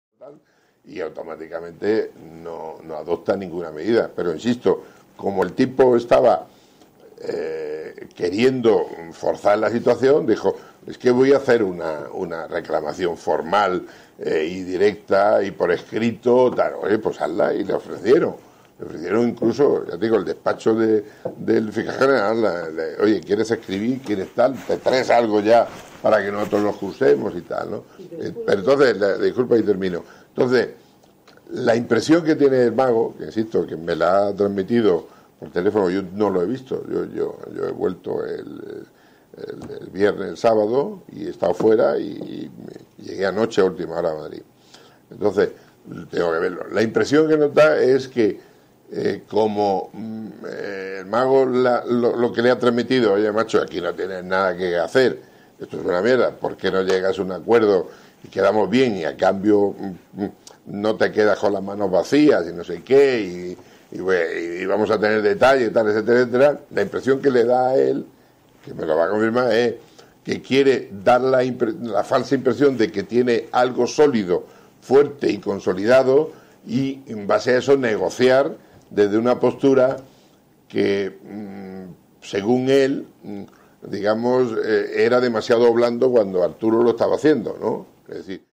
EXCLUSIVA | Villarejo afirma que Garzón y Delgado le ayudaron a parar la extradición de Pérez Maura
Audio 9. Mientras Garzón y Delgado refutan la veracidad del relato grabado de Villarejo, ninguno de los implicados rebate su existencia.